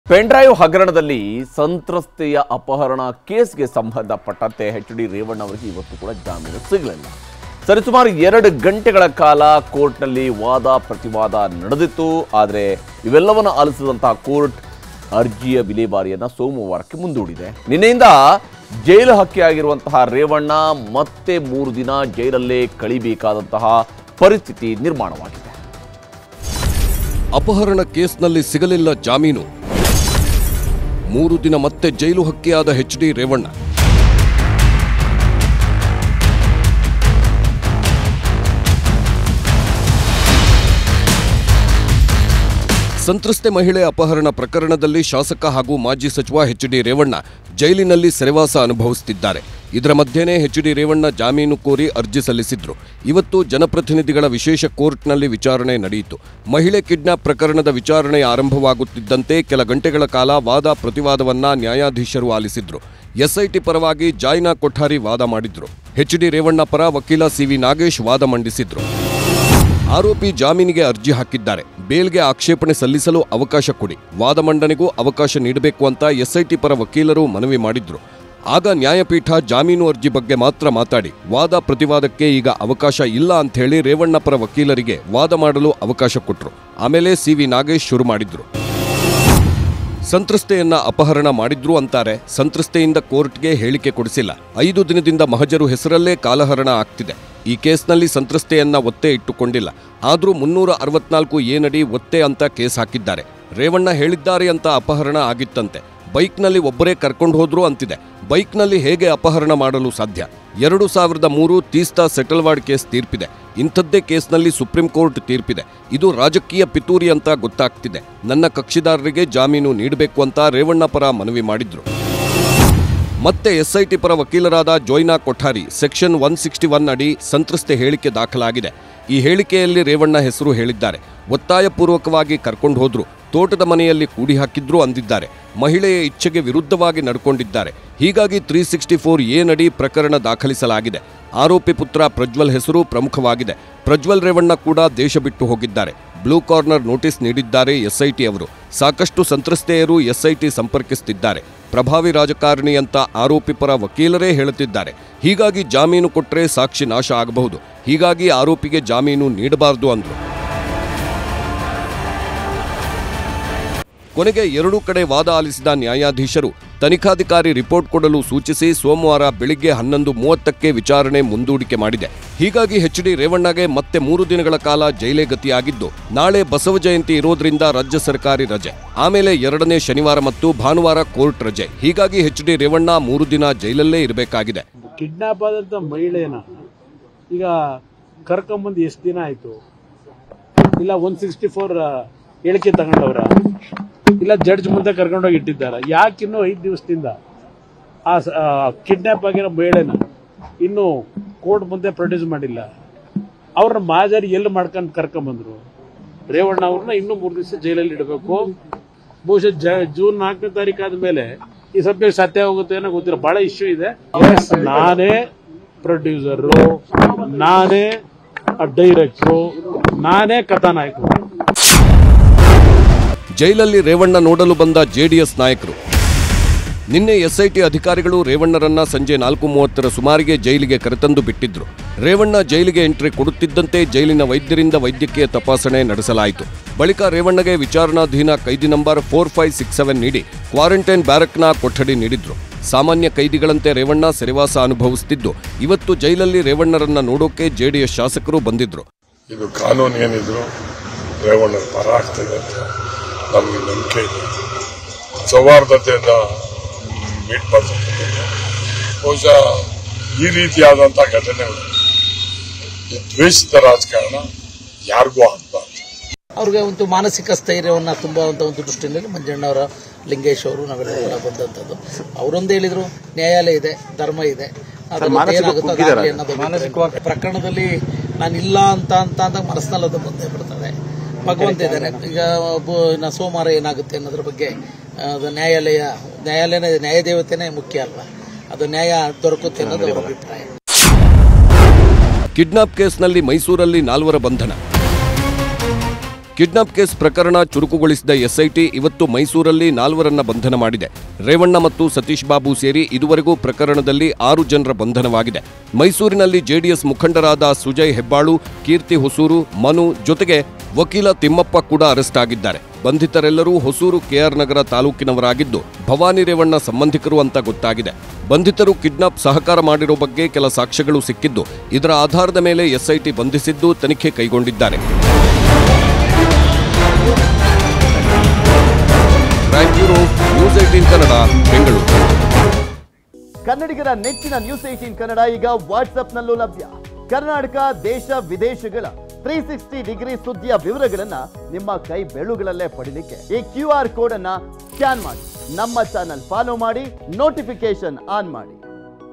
ಅಪಹರಣ ಕೇಸ್‌ನಲ್ಲಿ ಸಿಗಲಿಲ್ಲ ಜಾಮೀನು | HD Revanna Kidnap Case | Prajwal Pendrive Case
HD Revanna Kidnap Case | ಸಂತ್ರಸ್ತೆಯ ಅಪಹರಣ ಕೇಸ್‌‌ನಲ್ಲಿ ಹೆಚ್.ಡಿ ರೇವಣ್ಣಗೆ ಇವತ್ತೂ ಜಾಮೀನು ಸಿಕ್ಕಿಲ್ಲ.. ಸರಿಸುಮಾರು 2 ಗಂಟೆ ವಾದ ಆಲಿಸಿದ ಕೋರ್ಟ್‌ ಸೋಮವಾರಕ್ಕೆ ಅರ್ಜಿ ಮುಂದೂಡಿದೆ. ನಿನ್ನೆಯಿಂದ ಜೈಲು ಹಕ್ಕಿಯಾಗಿರುವ ರೇವಣ್ಣಗೆ ಮತ್ತೆ ಮೂರು ದಿನ ಜೈಲೇ ಗತಿ ಎಂಬಂತಾಗಿದೆ..

News18 Kannada Live | ನ್ಯೂಸ್ 18 ಕನ್ನಡ ಲೈವ್
Karnataka | ಕರ್ನಾಟಕ
Kannada | ಕನ್ನಡ